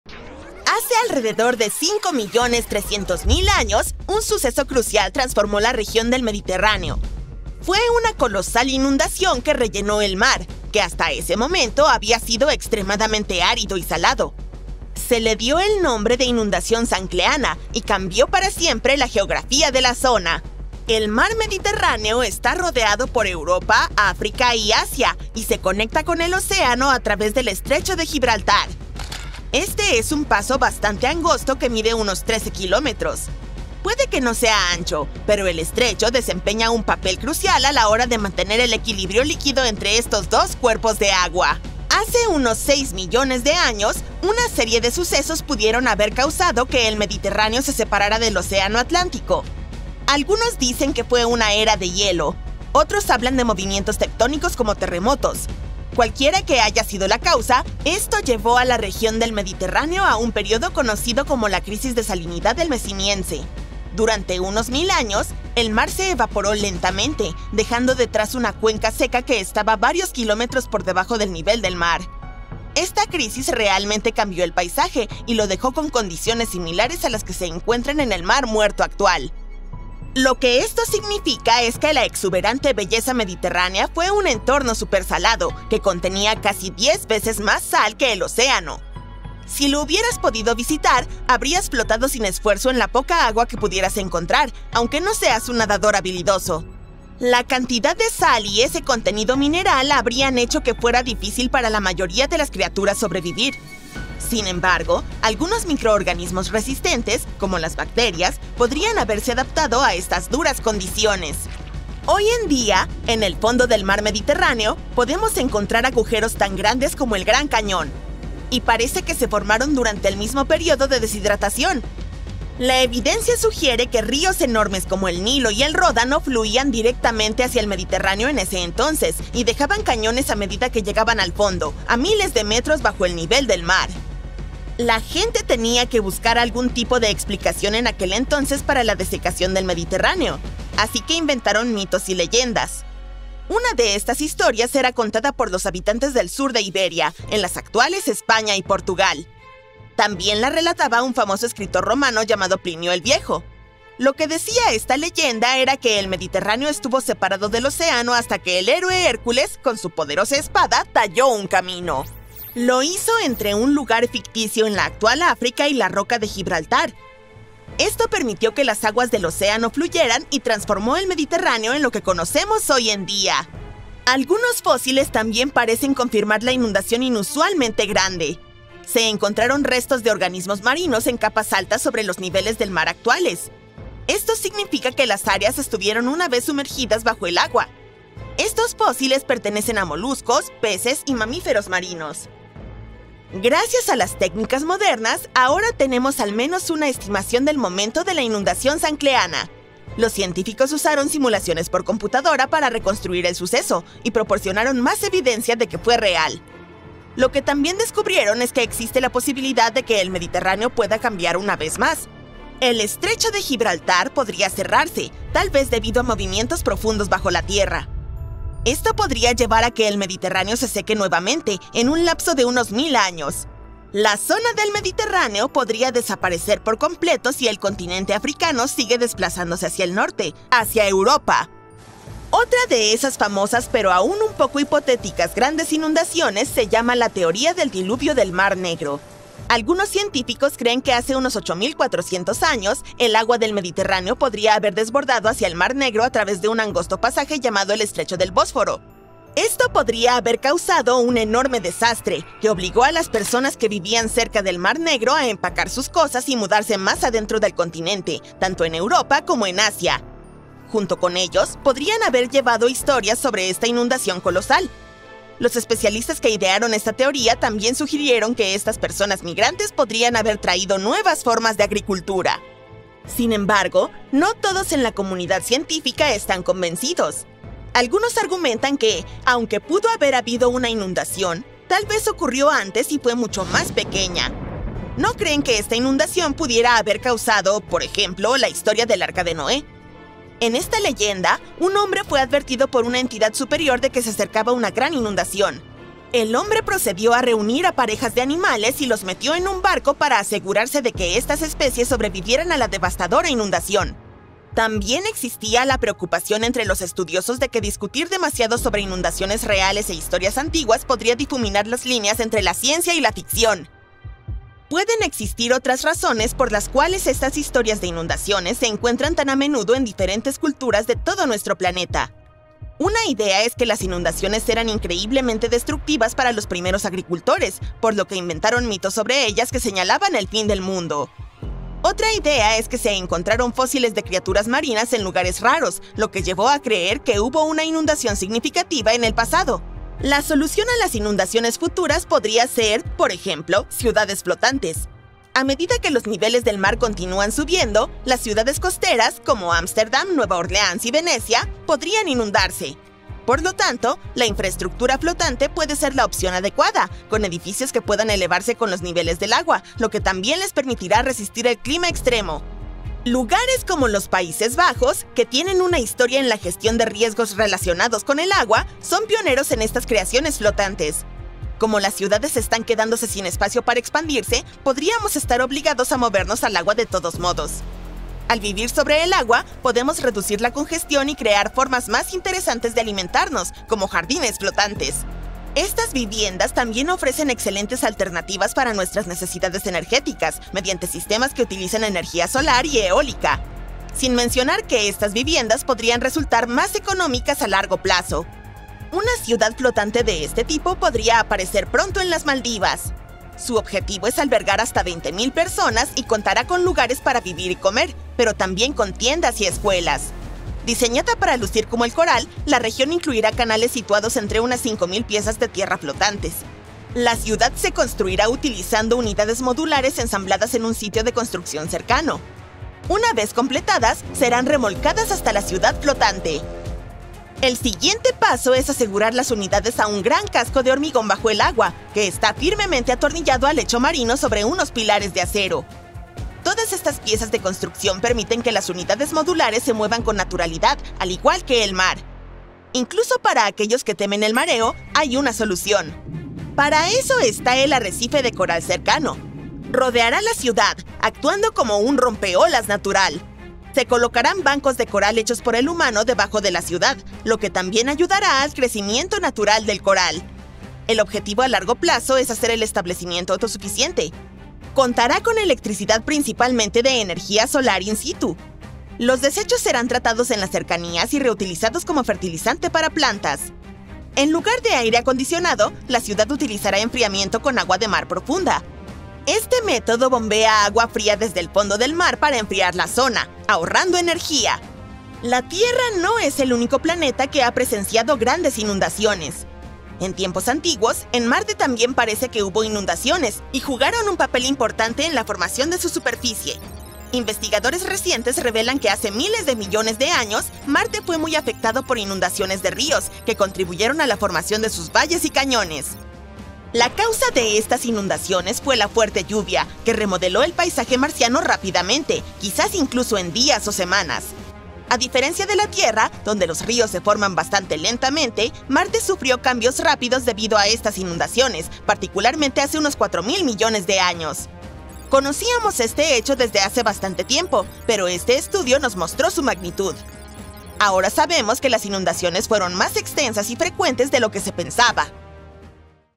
La megainundación que cambió la geografía de la Tierra para siempre
Imagina una inundación tan masiva que remodeló paisajes enteros. Eso es lo que sucedió con la megainundación durante la Edad de Hielo, conocida como las Inundaciones de Missoula. Enormes lagos glaciares en América del Norte se rompieron repentinamente, liberando agua que atravesó la tierra a velocidades increíbles. Esta poderosa fuerza esculpió cañones, creó vastos valles y dejó detrás enormes rocas esparcidas como guijarros. Fue uno de los eventos geológicos más dramáticos de la historia, cambiando para siempre la geografía de la Tierra.